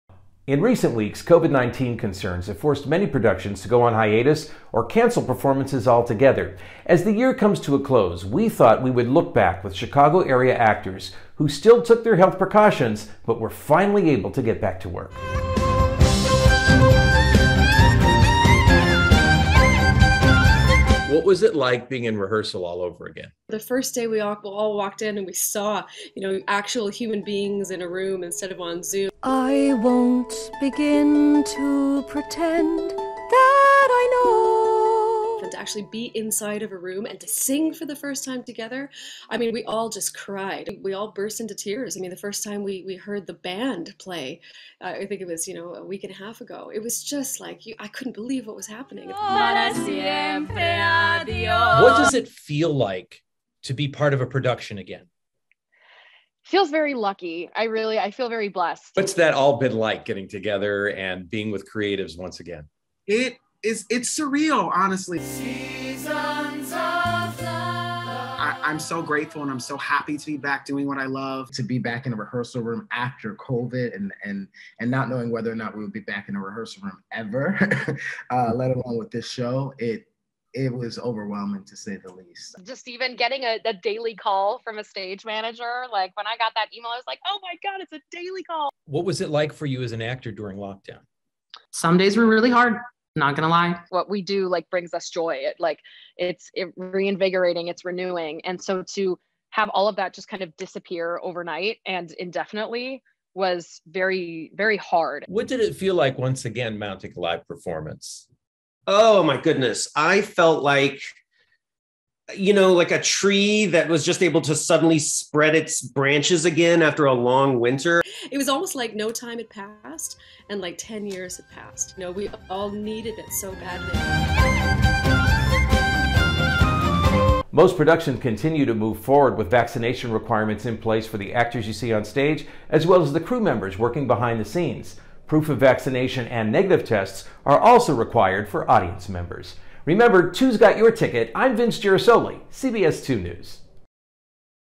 2's Got Your Ticket: A Look Back At COVID's Impact On Chicago Theaters
Actors and audiences returned to theaters in 2021. 2's Got Your Ticket, and entertainment reporter Vince Gerasole has reaction from those who were able to get back on stage and perform again.